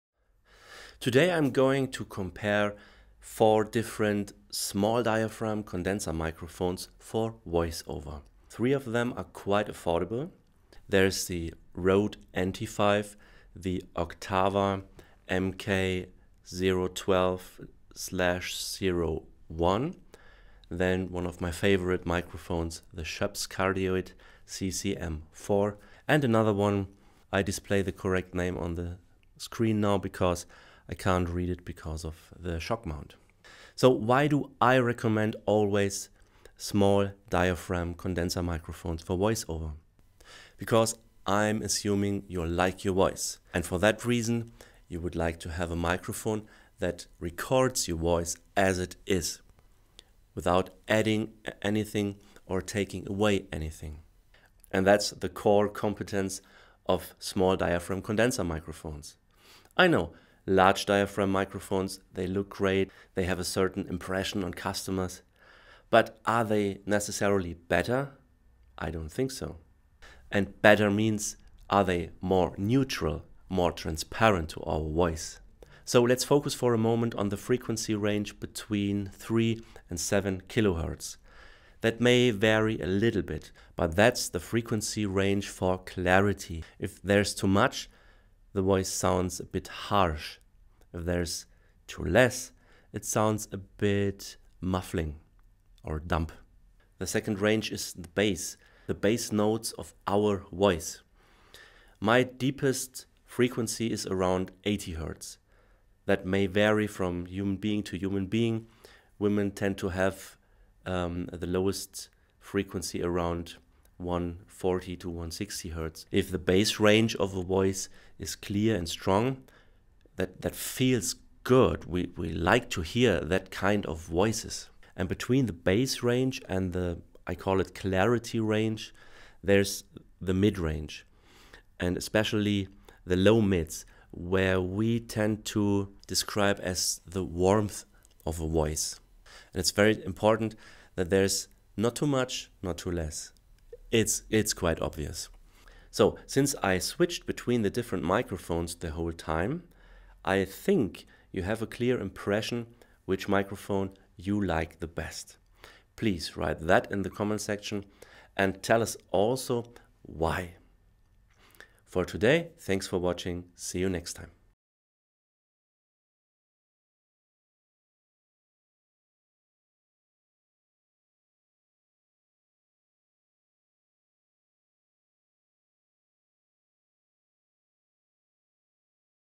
Rode NT5, Oktava MK 012-01, Schoeps CCM4 & Invotone CM 610 Pro | Comparison for VoiceOver [ENGLISH]
Recorded with a Schoeps CCM4, aRode NT5, an Oktava MK 012-01 and the Invotone CM 610 Pro. Recorder: ZOOM F6.